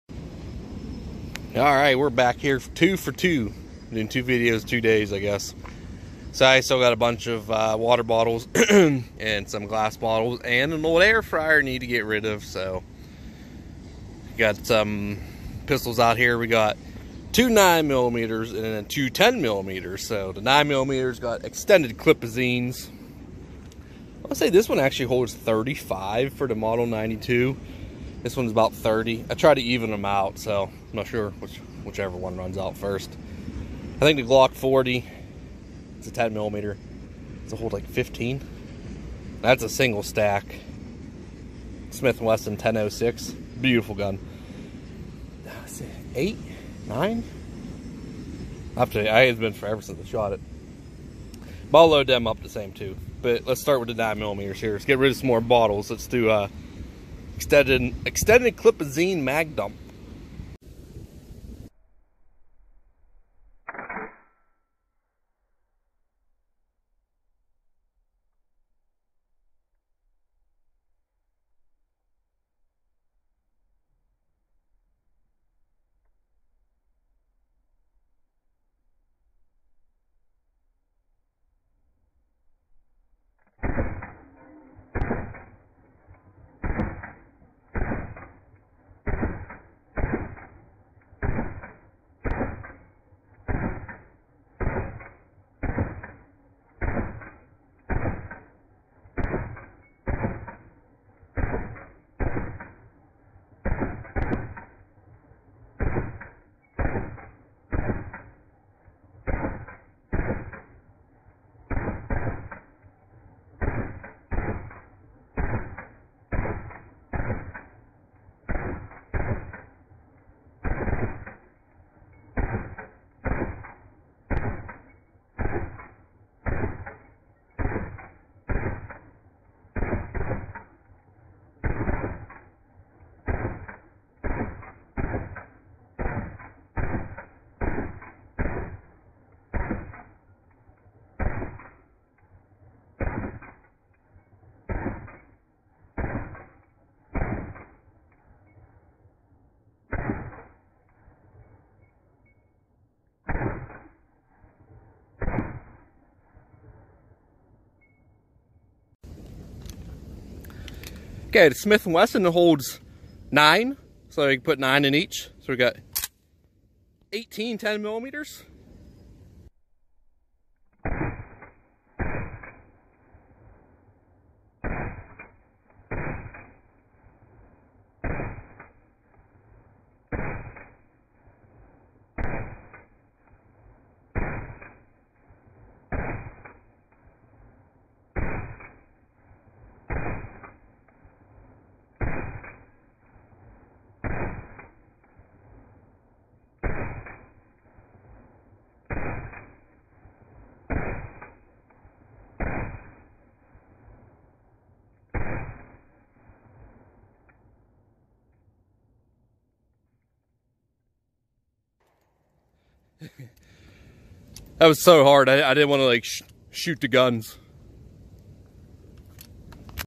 TWO 9MM AND 2 10MM !!! mag dumps
Doing a good old fashioned mag dump for trump 2020 brother